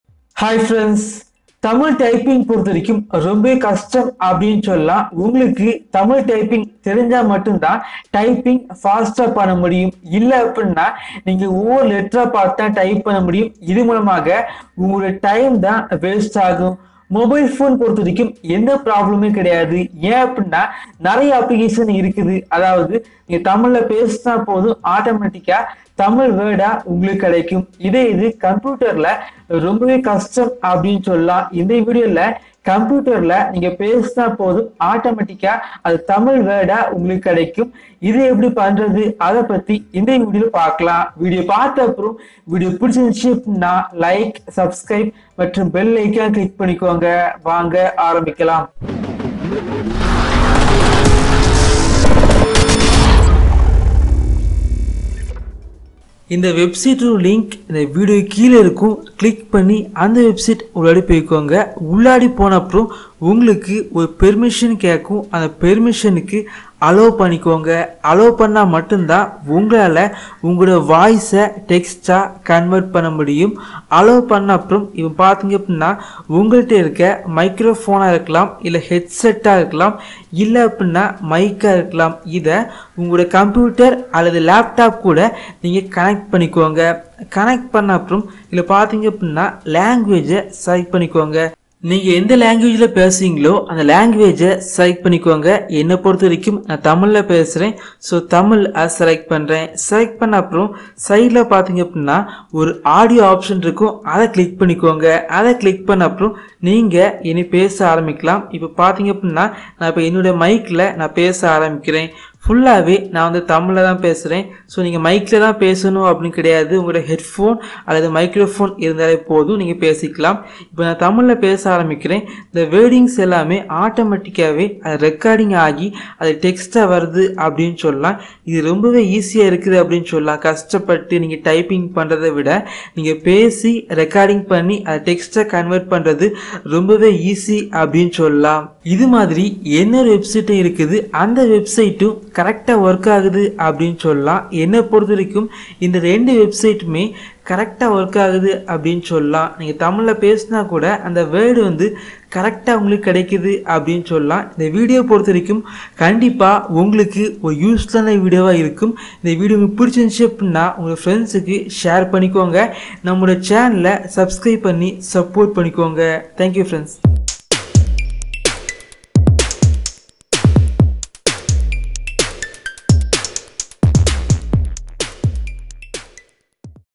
Tamil Voice Typing Software Free Download For PC - Computer Tips And Tricks In Tamil
Tamil speech to tamil text converter - Voice to Text convert websites is basically a speech recognition software tool that converts Tamil voice into Tamil text.